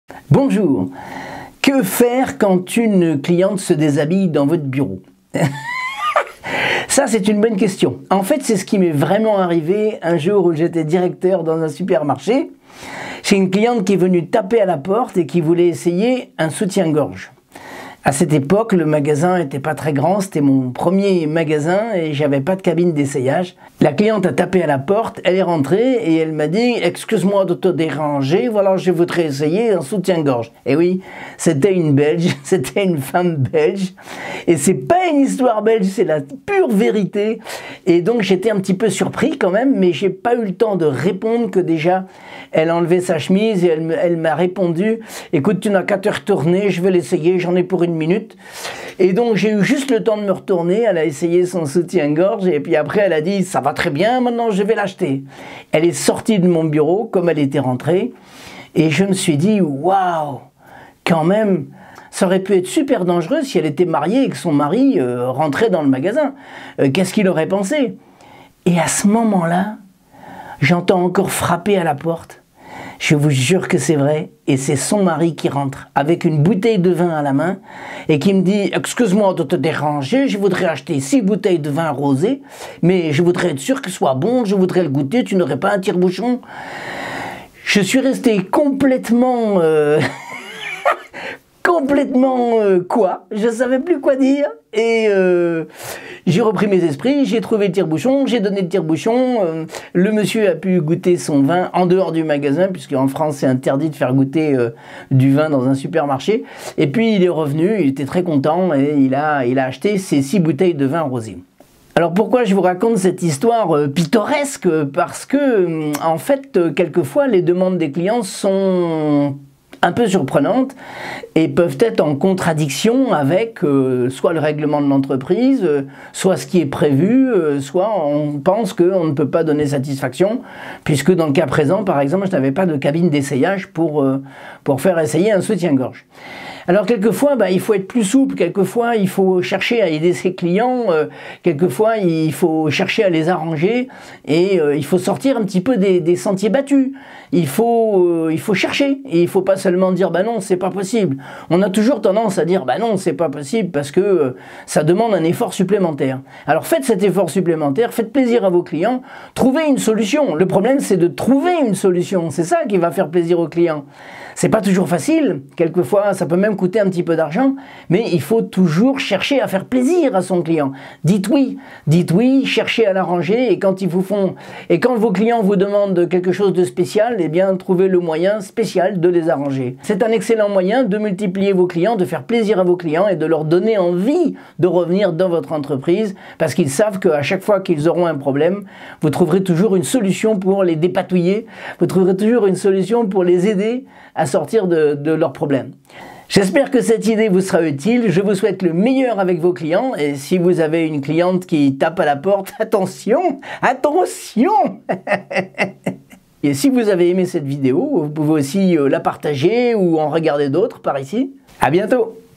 UNE VERITABLE HISTOIRE BELGE...par un conférencier professionnel expert du service client
Surprise totale lorsque je dirige depuis quelques semaines mon premier supermarché. Une femme frappe à la porte de mon bureau avec un soutien-gorge à la main et la surprise ne fait que commencer...par une conférencier professionnel expert du service client